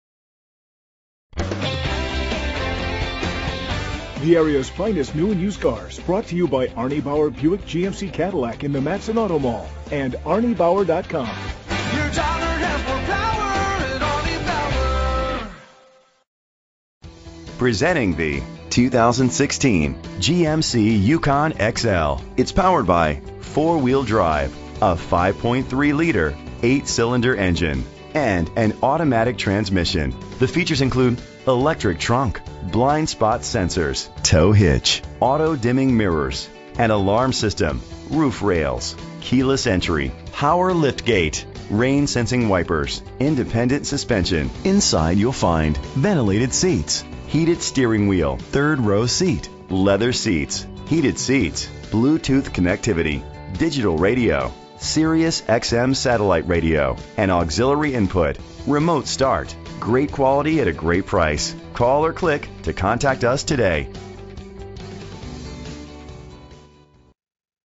2016 GMC Yukon XL G60322 - Matteson IL
Year: 2016
Make: GMC
Model: Yukon XL
Trim: SLT
Engine: 5.3 liter 8 Cylindercylinder 4WD
Transmission: Automatic
Color: Off White
Mileage: 15
Address:
5525 Miller Circle Drive
Matteson, IL 60443
VIN:1GKS2GKC7GR367720